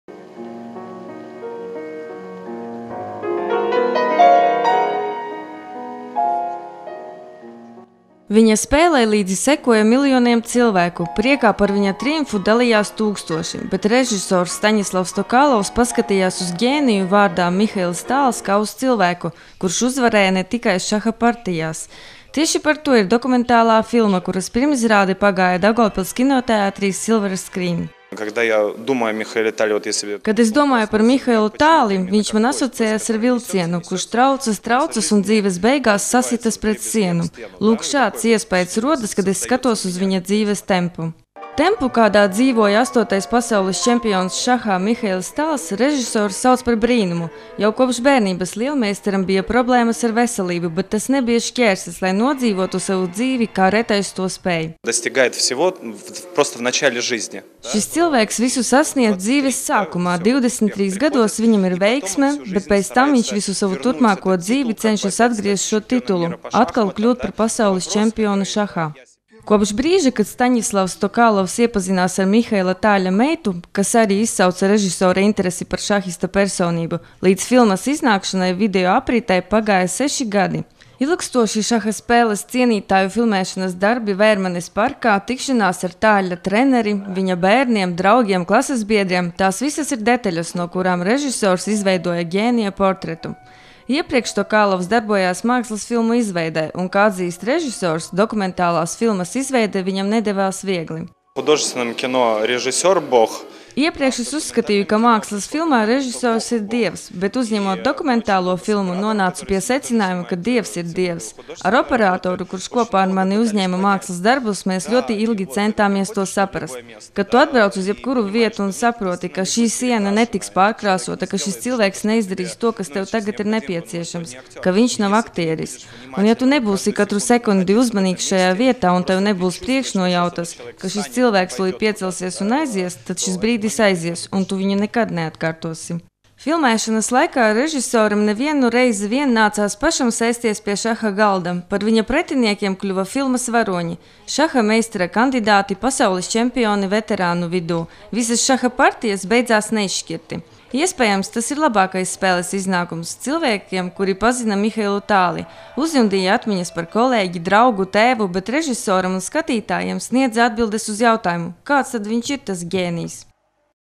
Mihailam Tālim šogad apritētu 80 gadi
Pirms šahista jubilejas kinoteātru ekrānos iznāca Latvijas režisora Staņislava Tokalova dokumentālā filma "Laiks, kura nav. Mihails Tāls", Dautkom TV filmēšanas grupa devās uz filmas pirmizrādi.